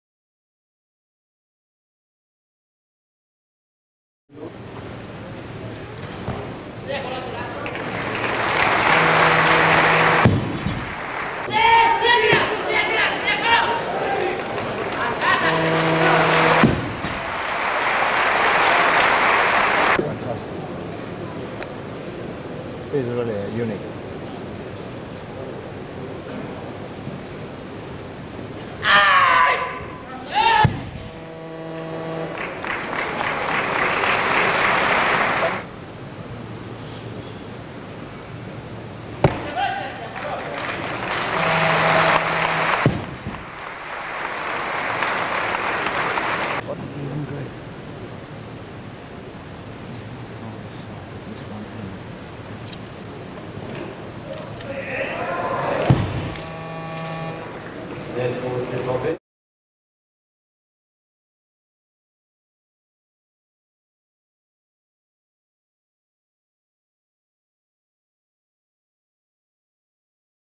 Czanka Attila | World Weightlifting Championships | 1986 | 60kg
Az ekkor még román színekben induló kolozsvári születésű Czanka Attila az 1986-os világbajnokságon a 60kg-os súlycsoportban versenyzett. 130kg-os szakításával és 160kg-os lökésével összetettben (290kg) a második helyet szerezte meg.

Érdemes szétnézni a csatorna videói között, rengeteg érdekesség akad. Napi érdekességekért keresd fel a oldalt.